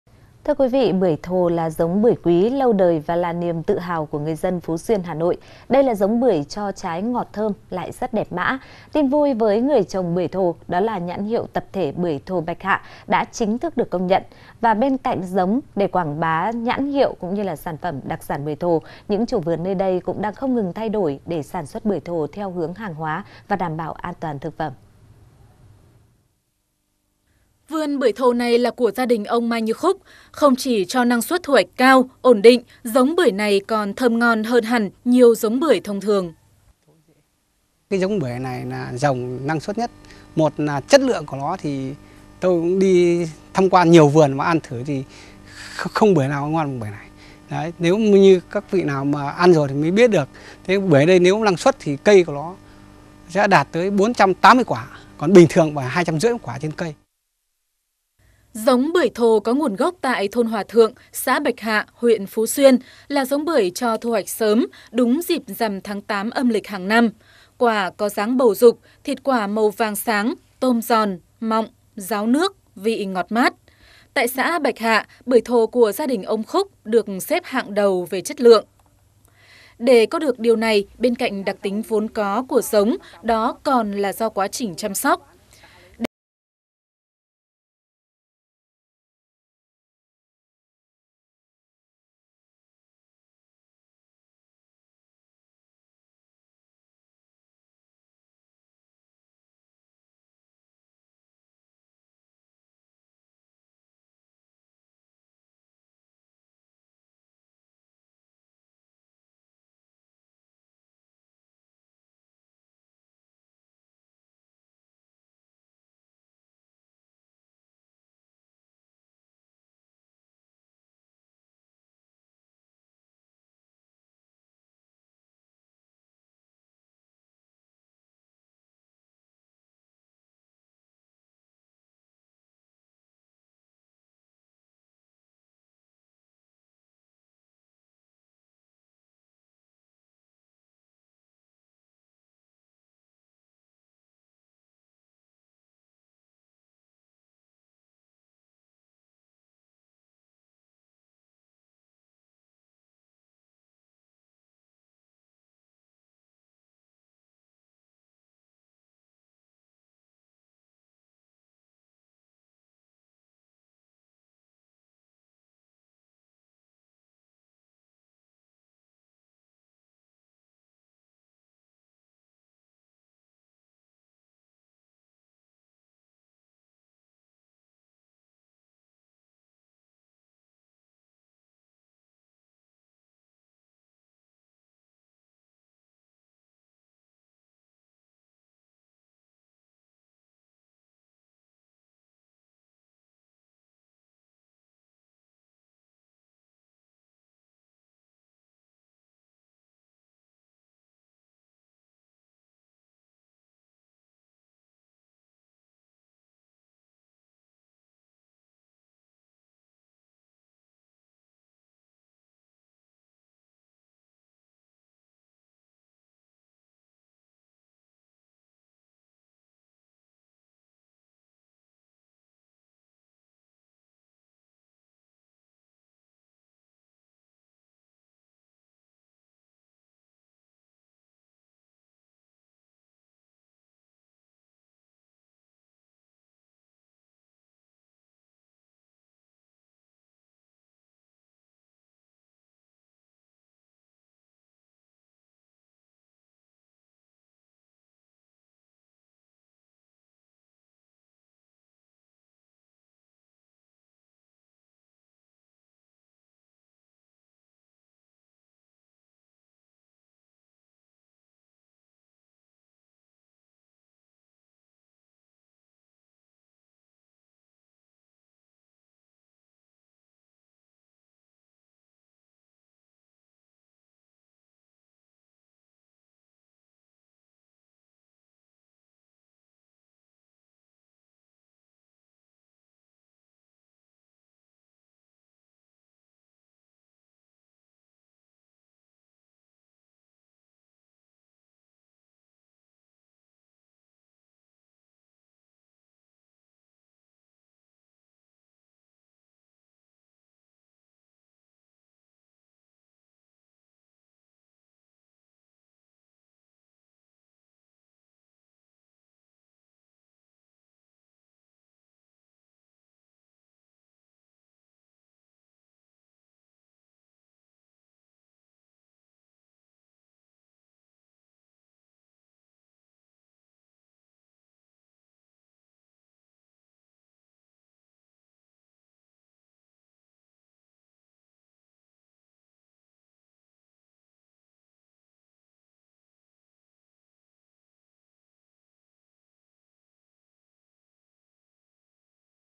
Bưởi Thồ quý hiếm, trồng không đủ bán | VTC16
VTC16 | Nhãn hiệu tập thể bưởi Thồ Bạch Hạ đã chính thức được công nhận. Không chỉ ngọt thơm mà trái bưởi Thồ còn rất đẹp mã. Nhờ trồng theo hướng an toàn, người trồng bưởi Thồ ở Phú Xuyên - Hà Nội không còn lo lắng về đầu ra.